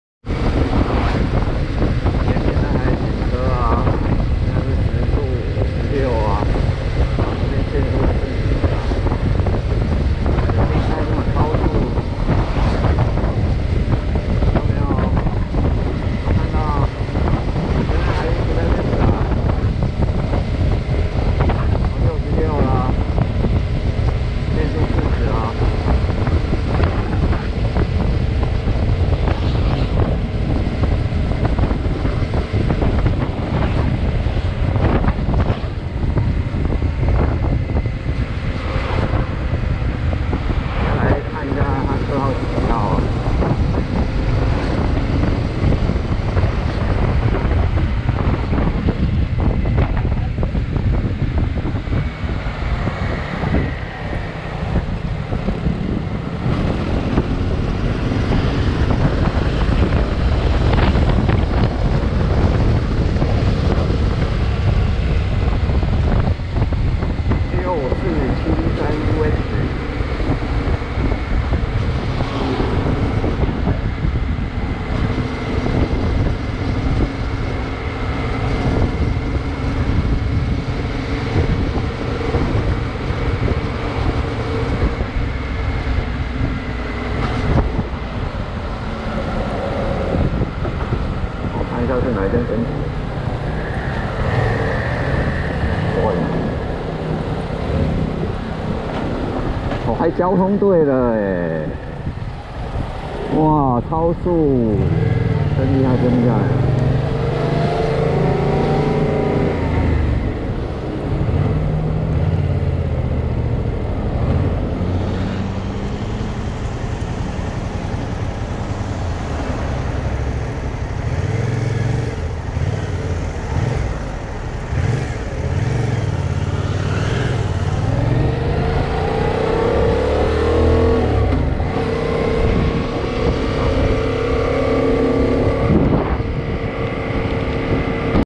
阿Sir 您超速了
這條路在尖峰時間.總有警察拿著三眼狙擊用路人.結果看看自己人已經刻意壓低速度還是超速.這速限合理嗎?在幾乎封閉的路面限速40KM/HR .這不叫擾民
甚麼才叫擾民.執法機關往往為了完成上頭交代的業績.故意挑低肇事卻易違規的地點來選擇執法.完全背離了當初執法的初衷.執法的用意是為了保障用路人的安全.現在卻變成了把人民當提款機.這樣的情況屢見不鮮.立法機關常拿著幾十年前訂定條件基準.套在現行的路況條件上.畏事怕事標準的公務員心態.難怪
越來越多爭取路權改善的活動一再的發起.